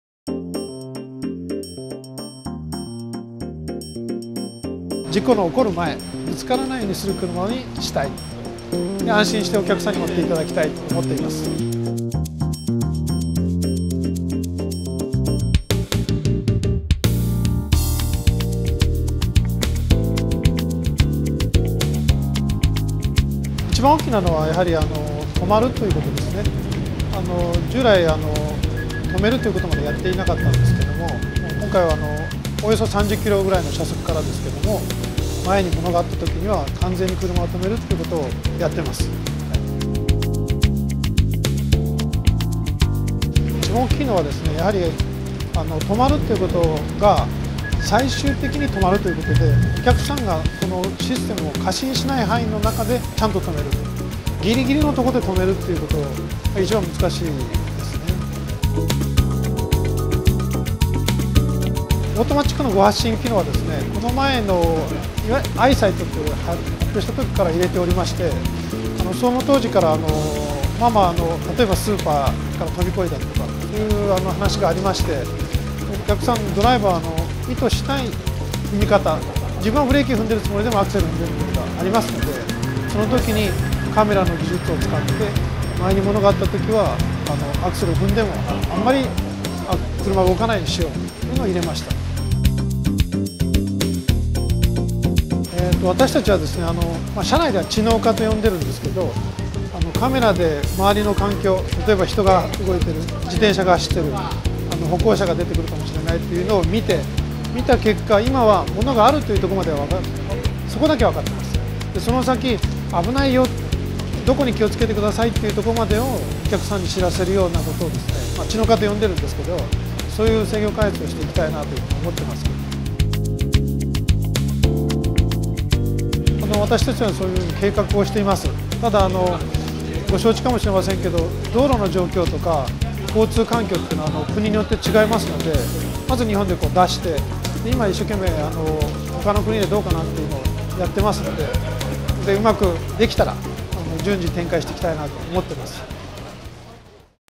スバル 新型 EyeSight - Subaru New EyeSight
スバル新型EyeSight   2010年4月22日発表会場にて撮影
株式会社カービュー